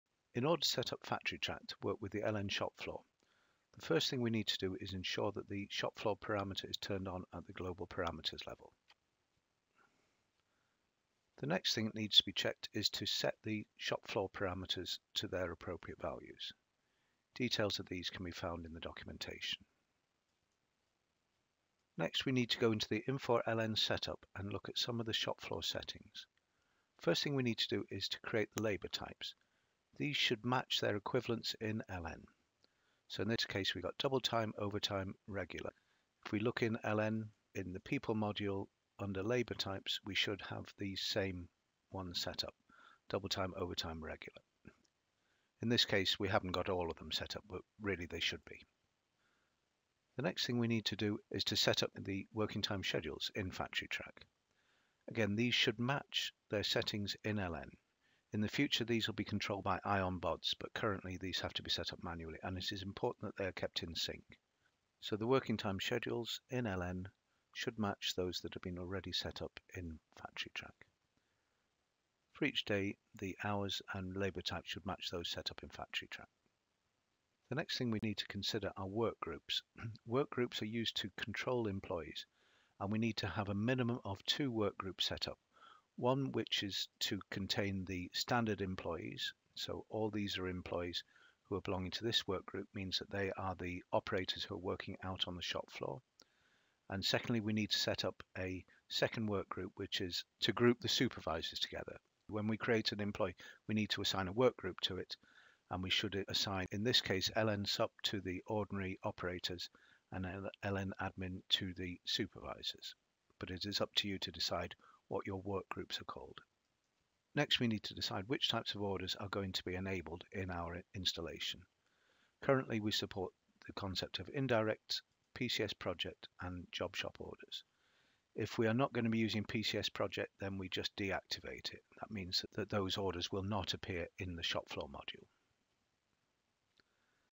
LN Shop Floor Setup Step 2 - Initial Setup
First Steps Required to Configure Infor Factory Track for LN Shop Floor